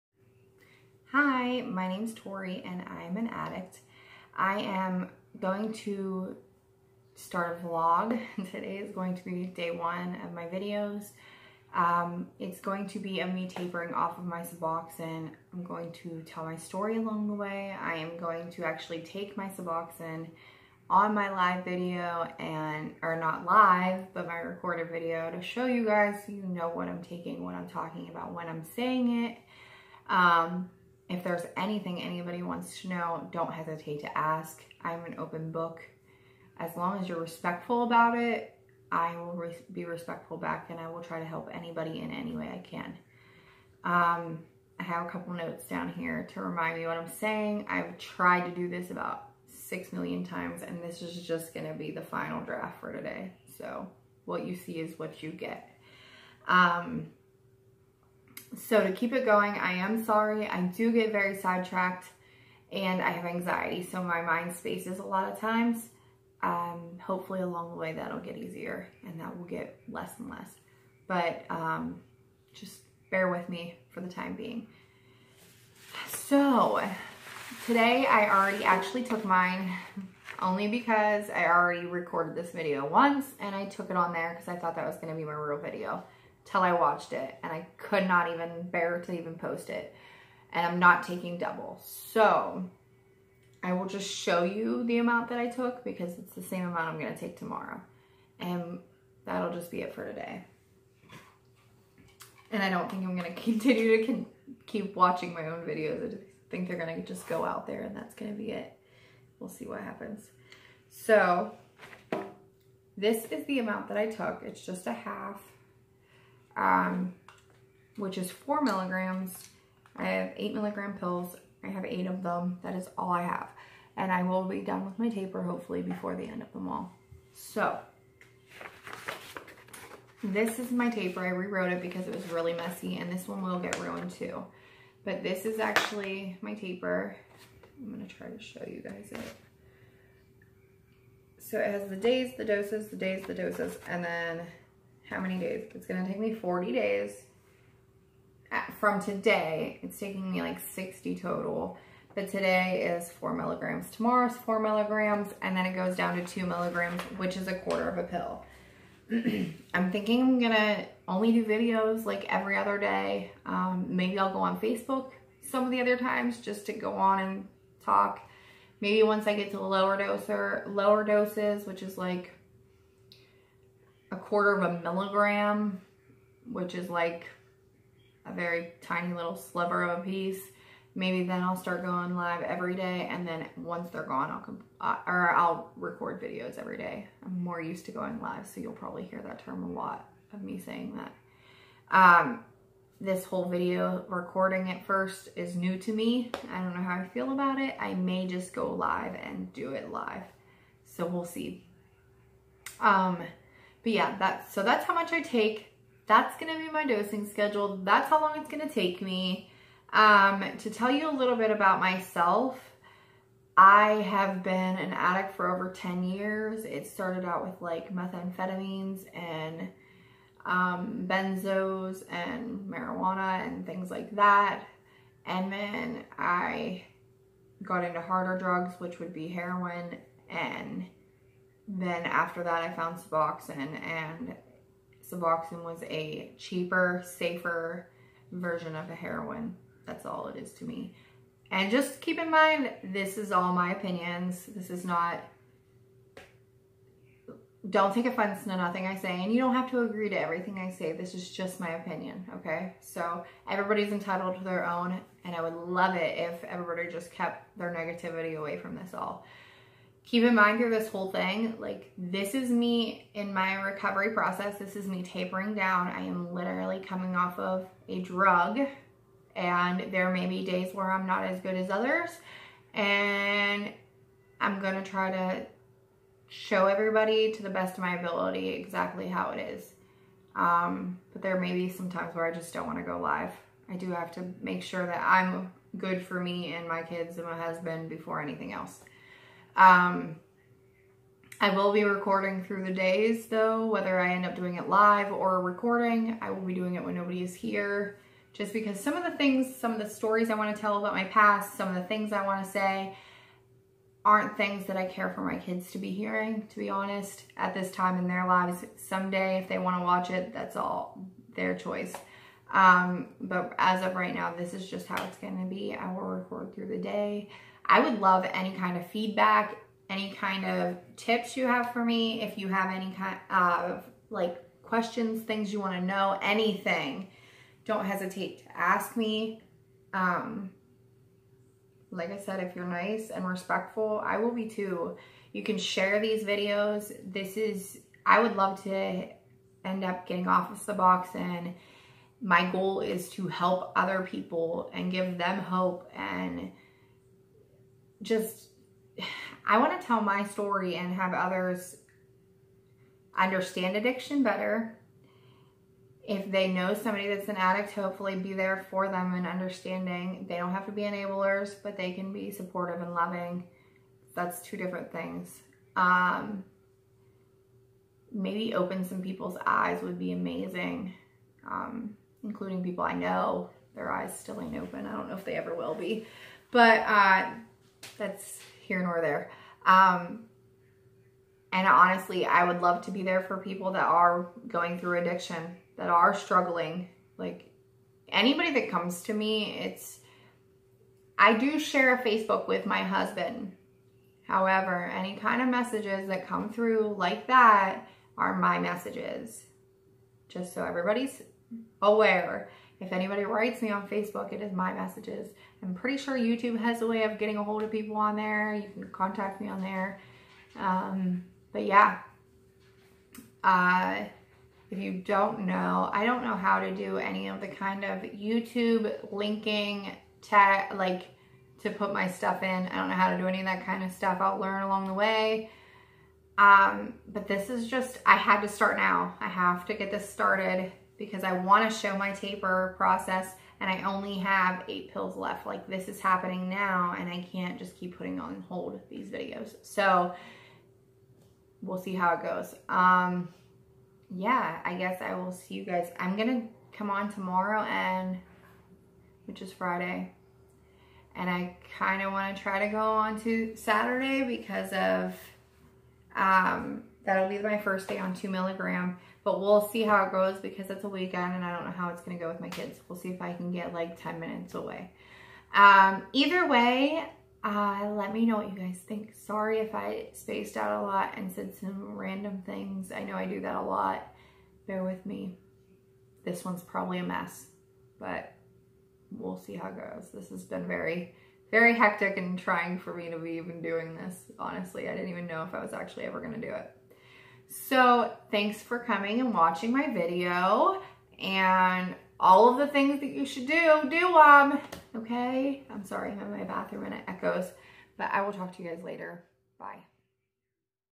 First day on taper vlog
Hey everyone!! Today is my first day.doing my vlog taper. Please have patience with me as I figure out how to do these videos.
Right now I am on 4 mg which is a half of a pill and in two days will be down to 2.mg.
I am here to help anyone that's struggling with addiction or  knows someone that is.
Please no negativity ..not just to me but anyone that wants to share here!!.